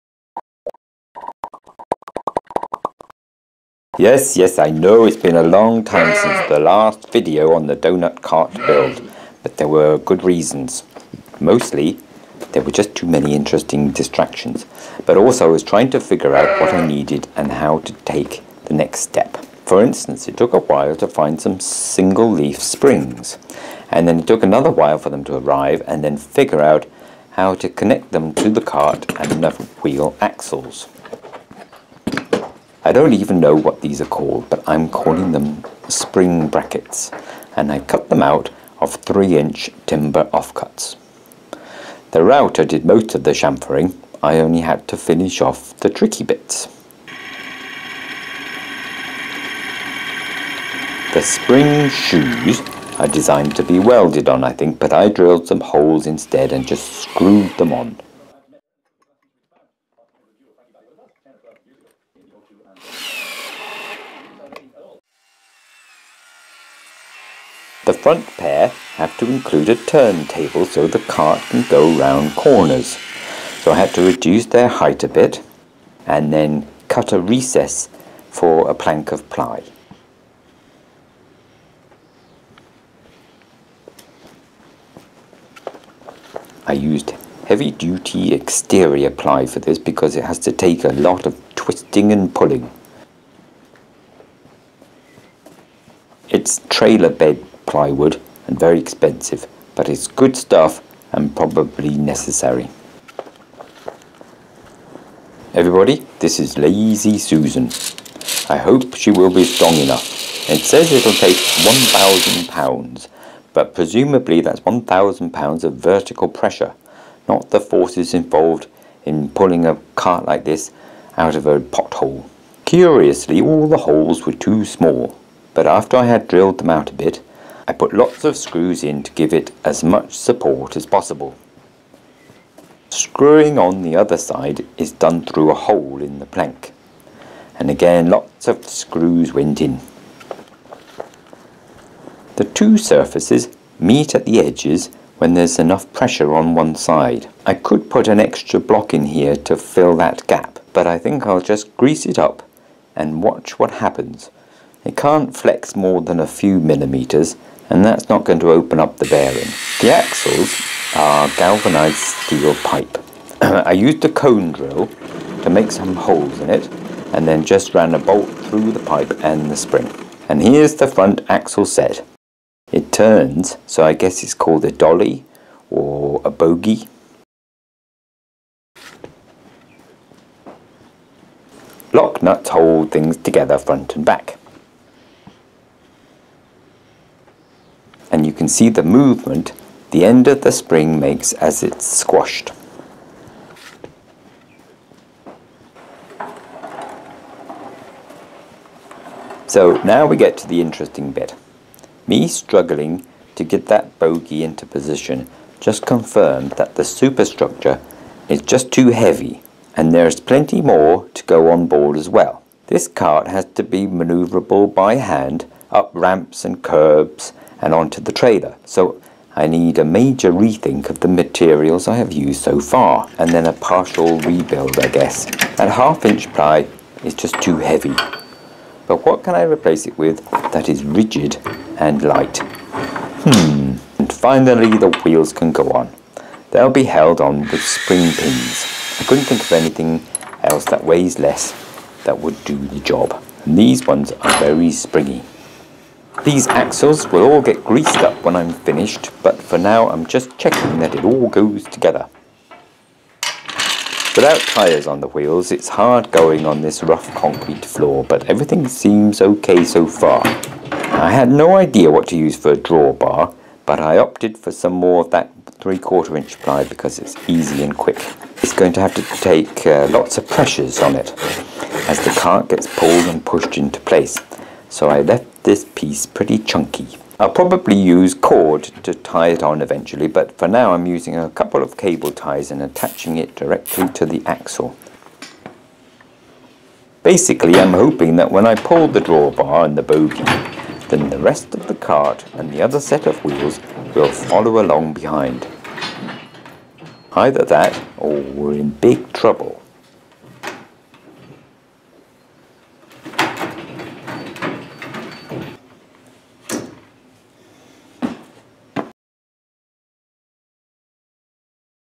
Clockwork Doughnut Cart: Part Four - The Chassis
Lots of people have reminded me that the doughnut cart is still not finished – thank you.   In fact, I walked past it everyday so I already knew that! Finally back working on it now and it’s great. I love it really, even though the weight issue is a bit of a challenge. How to reduce the weight and still maintain rigidity. That’s the challenge for next time..